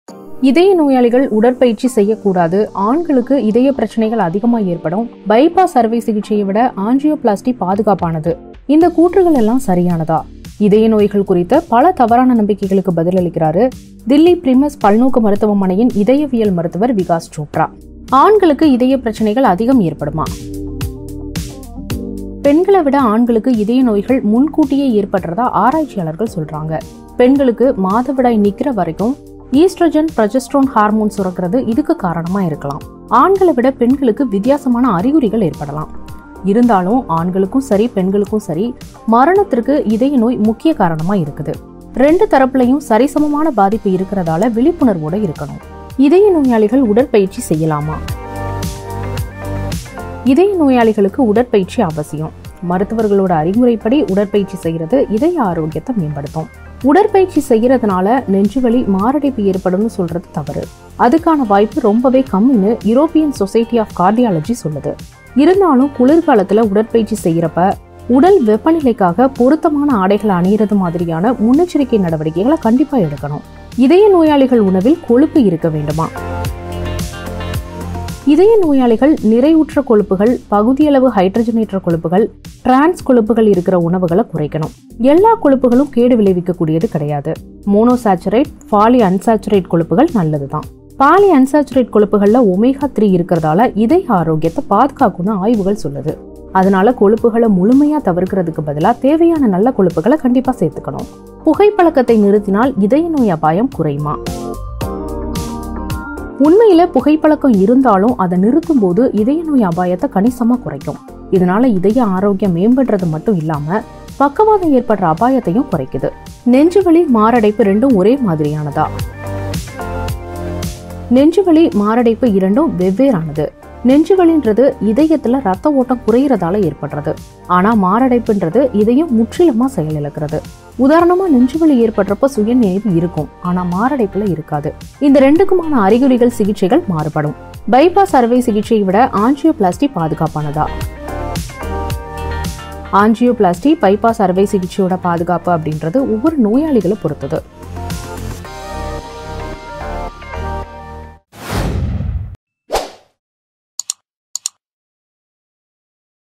இதய நோயாளிகள் உடற்பயிற்சி செய்யலாமா? - நம்பிக்கையும் உண்மையும்! | Dinamani
Dinamani Daily Seithigal Morning:
Dinamani Daily Seithigal Evening:
Dinamani Daily Breaking News:
Hourly Headlines: